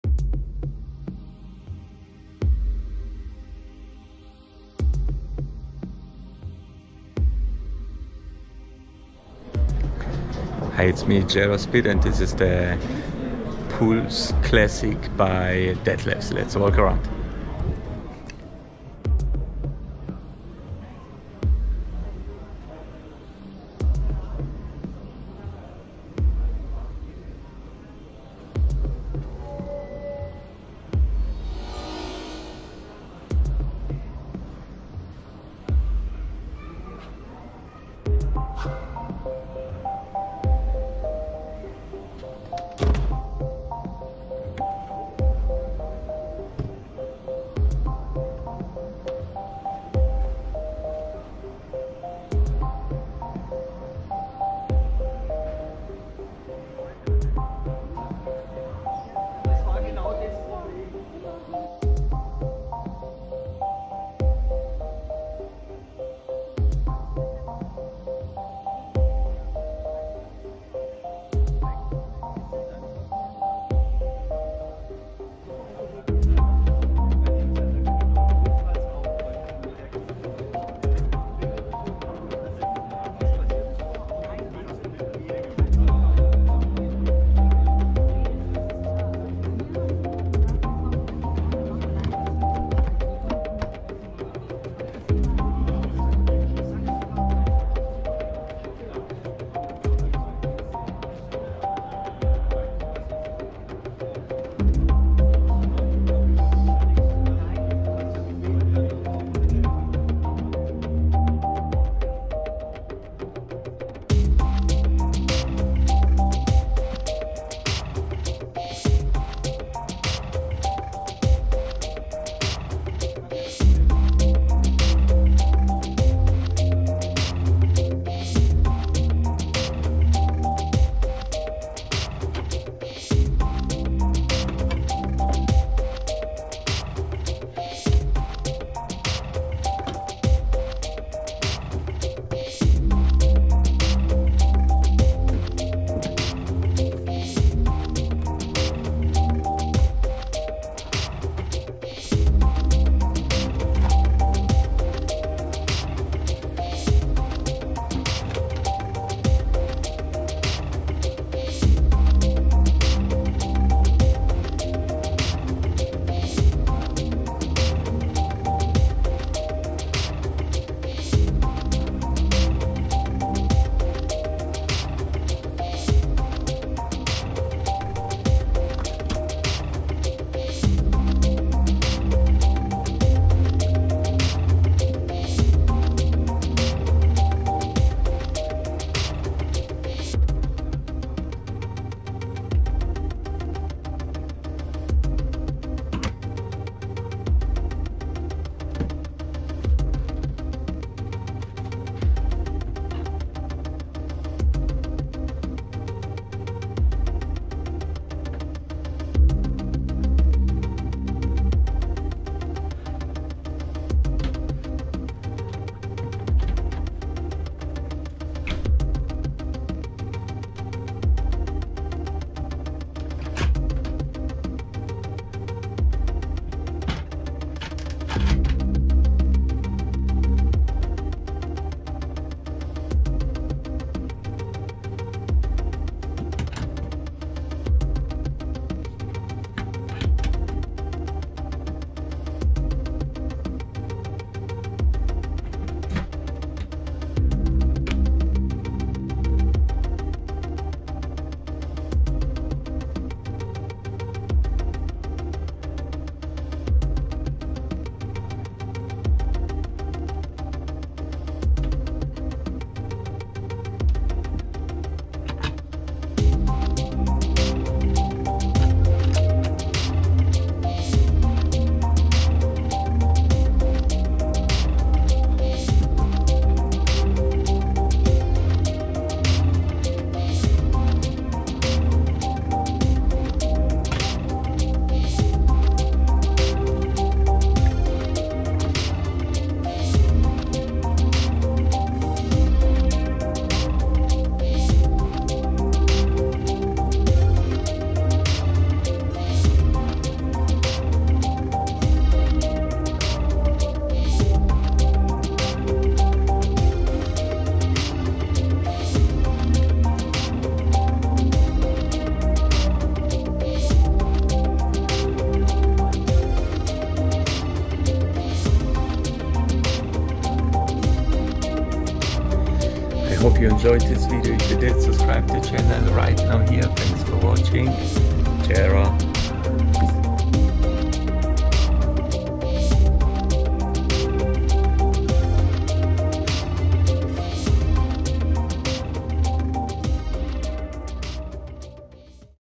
Dethleffs Pulse Classic T 7051 EBL RV Camper Van Fiat Ducato model 2020 walkaround + interior K0232
jaro speed
Dethleffs Pulse Classic T 7051 EBL
Fiat Ducato 2,3L / Mjet / 103kw / 140hp
L:741 / W:233 / H:300 / weight: 3068kg
total price of exhibited vehicle in this configuration on this exhibition was: 74.334.-EUR
2020